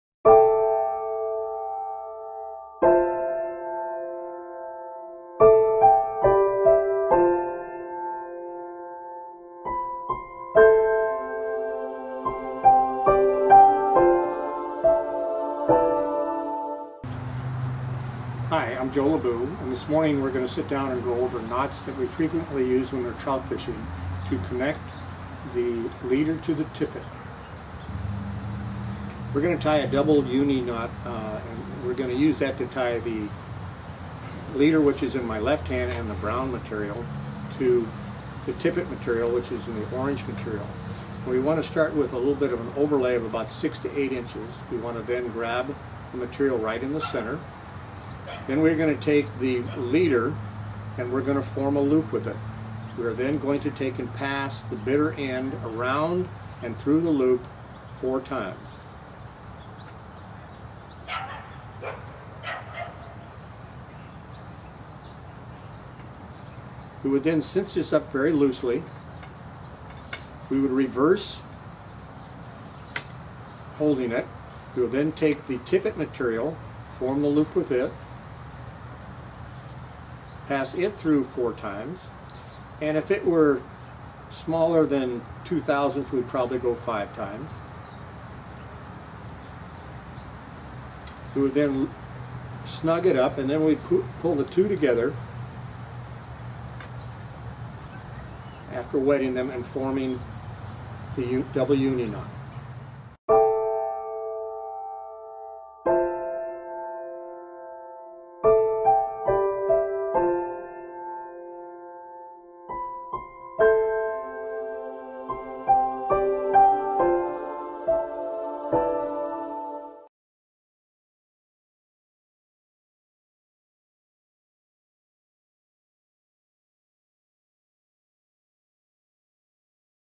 Joe Libeu Ties the Double Uni Knot
Joe LIbeu, Professional Flyfishing Guide and Master Fly Casting Instructor, ties the Double Uni Knot, often a critical part of the fly leader link to the fly.